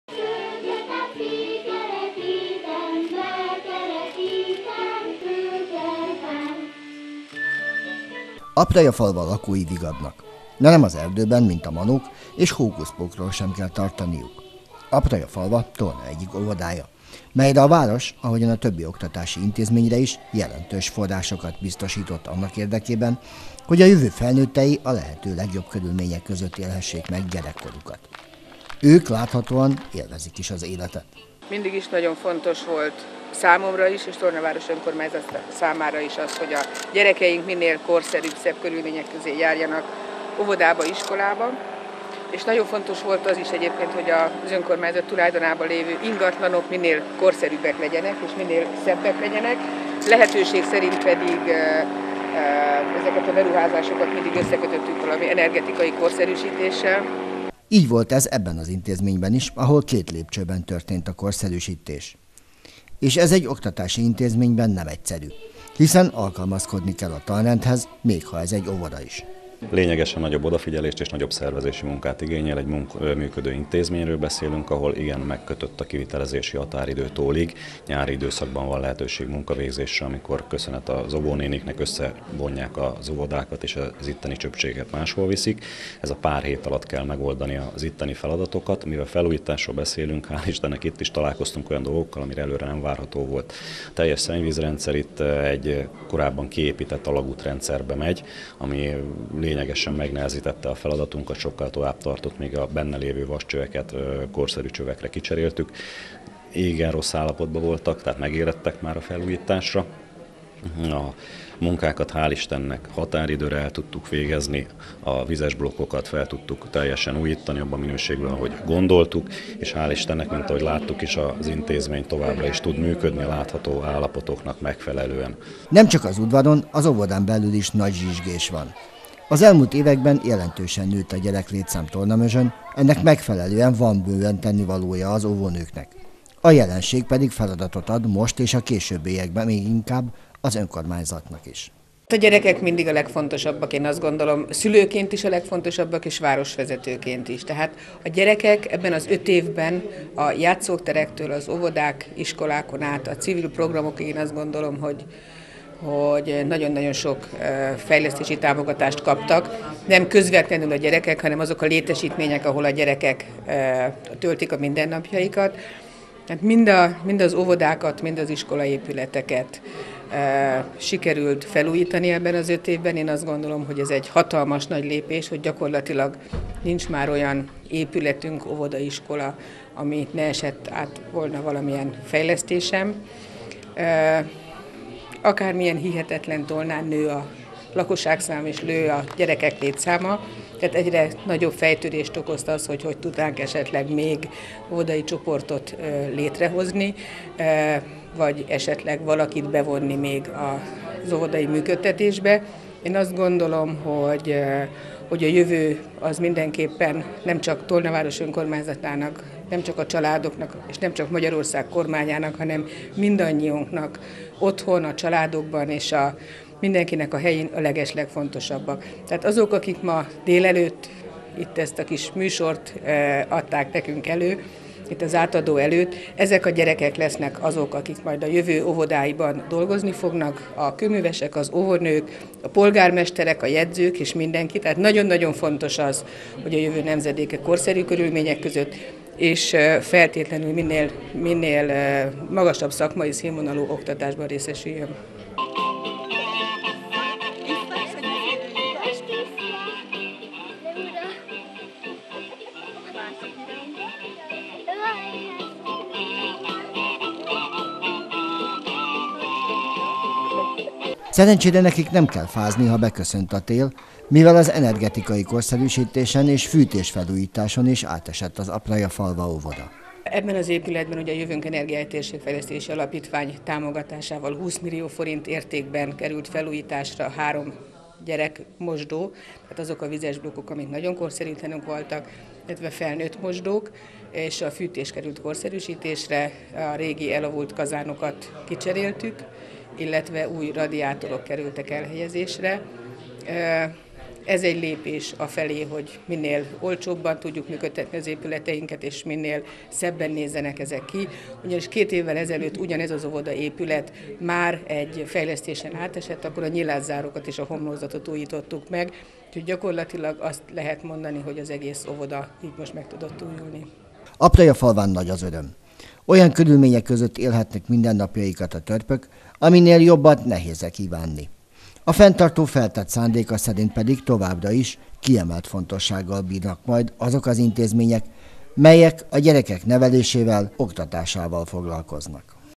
Aprajafalva Óvoda felújítás - Tolna 20190925
Az elmúlt 5 esztendőben Tolnán kiemelten kezelték a gyerekek nevelésével foglalkozó intézmények korszerűsítésének ügyét. Ennek megfelelően minden oktatási intézményben történt, elsősorban energetikai korszerűsítés. Így volt ez az Aprajafalva Óvodában is, ahol két ütemben korszerűsödött az intézmény.